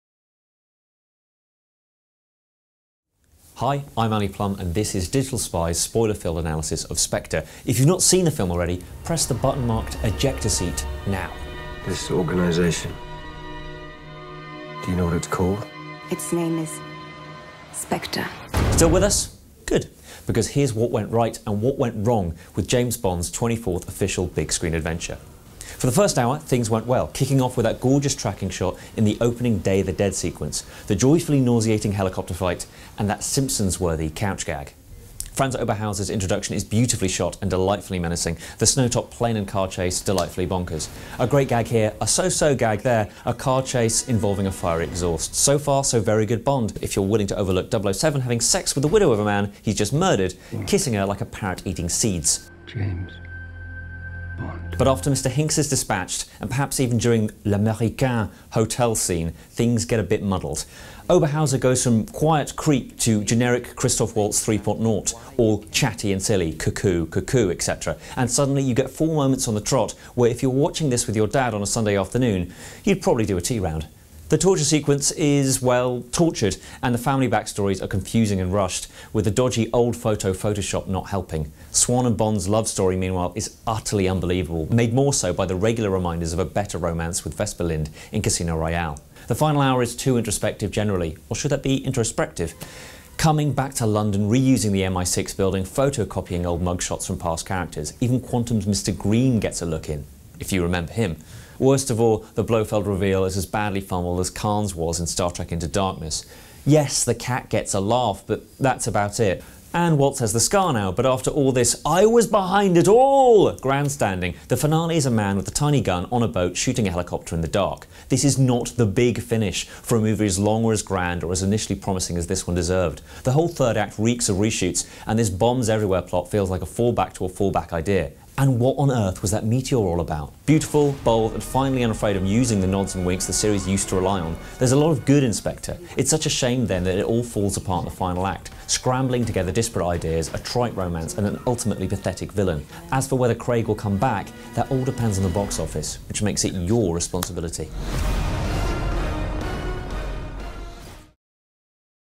James Bond 'Spectre' spoilerific review
Read the title - SPOILERS AHEAD. Digital Spy movie buff Ali Plumb gives his verdict on James Bond's latest adventure 'Spectre'.

Synopsis: A cryptic message from Bond's past sends him on a trail to uncover a sinister organization. While M battles political forces to keep the secret service alive, Bond peels back the layers of deceit to reveal the terrible truth behind SPECTRE.

'Spectre' stars Daniel Craig as James Bond in his fourth outing as the spy. Joining him are Christoph Waltz (Django Unchained), Léa Seydoux (Blue Is The Warmest Colour), former WWE wrestler Dave Bautista (Guardians of the Galaxy) and Skyfall returnees Naomie Harris, Ralph Fiennes and Ben Wishaw.